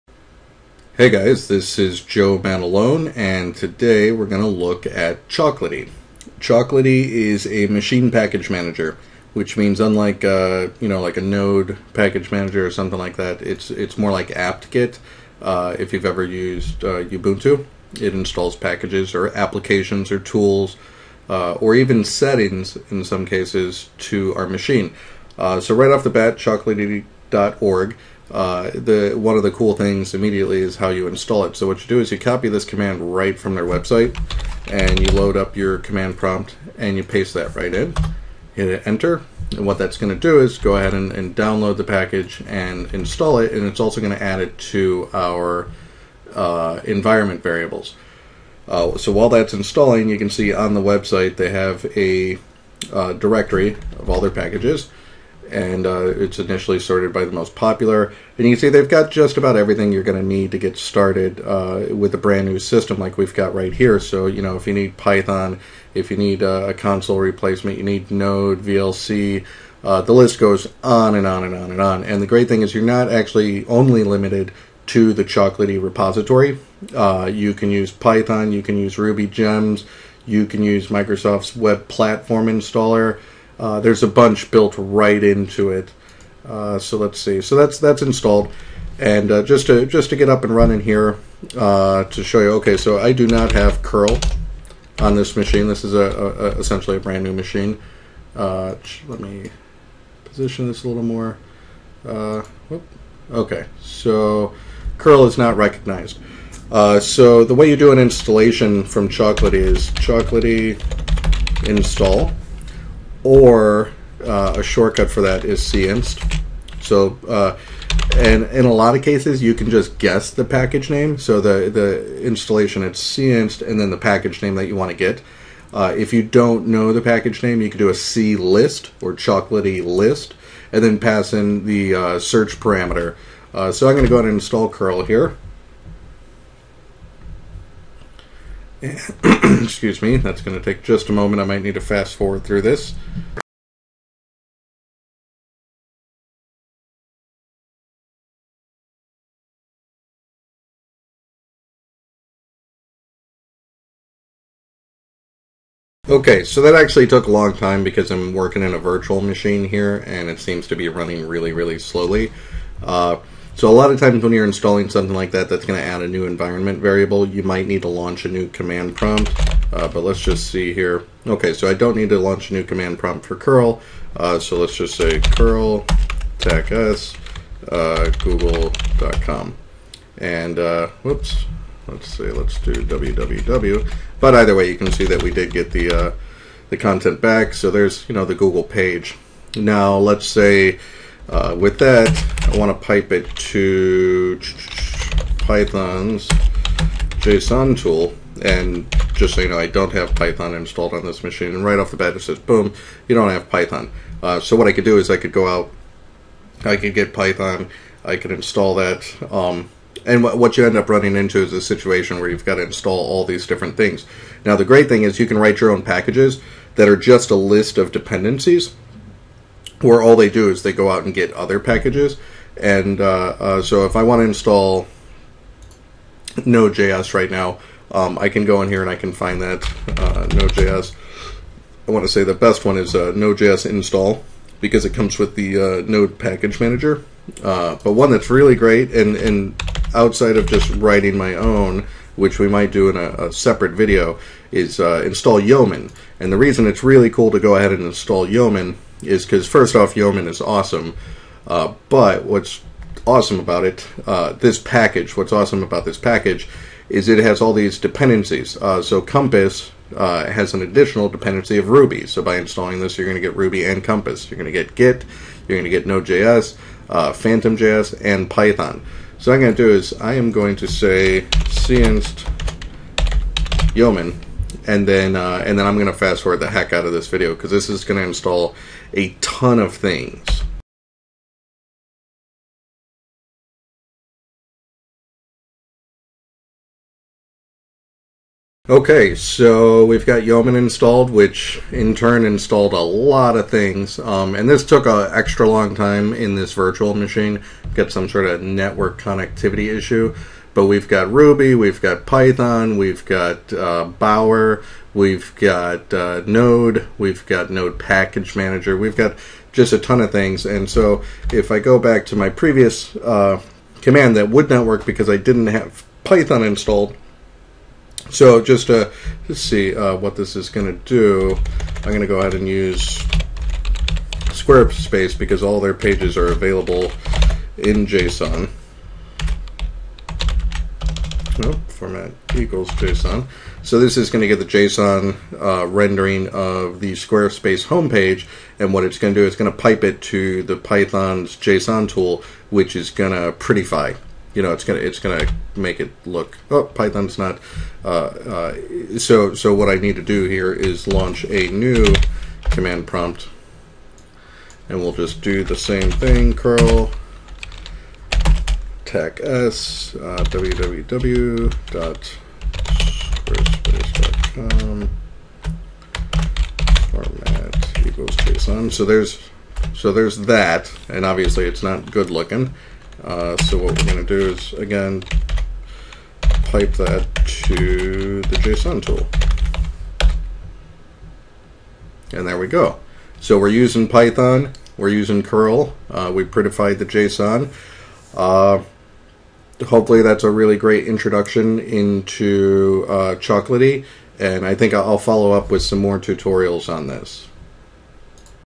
Chocolatey Introduction
Installing the Chocolatey Machine Package Manager.  Then installing nodejs, python, ruby, curl, bower, and Yeoman to Windows from the the command line.